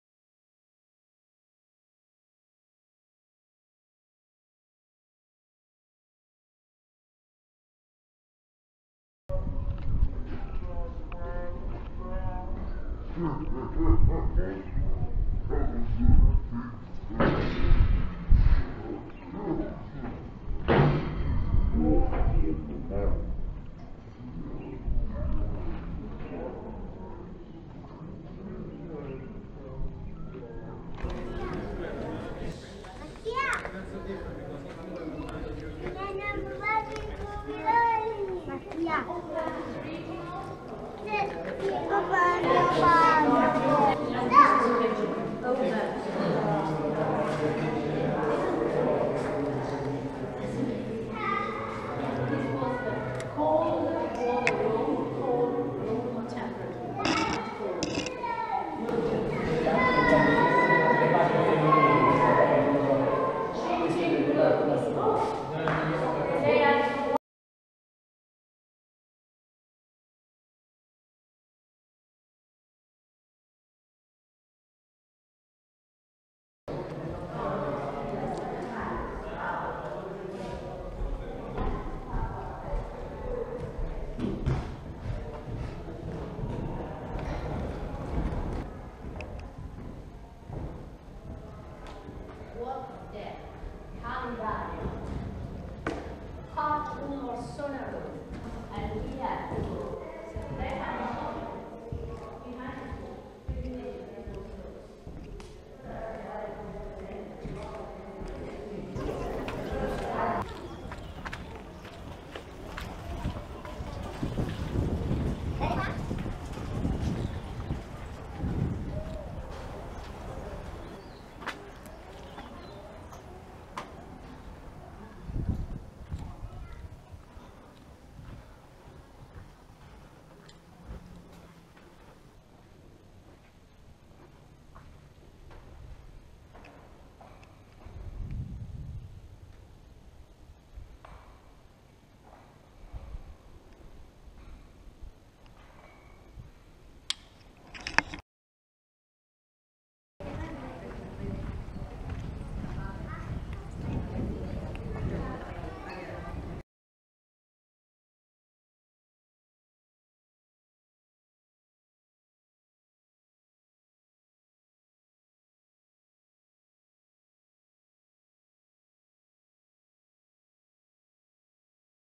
Thermes de Stabies
Les Thermes de Stabies sont les plus anciens de Pompéi. Les bâtiments se divisent en deux sections distinctes, l'une pour les hommes et l'autre pour les femmes. Cette séquence fait partie de nos articles sur la visite détaillée du site par quartier.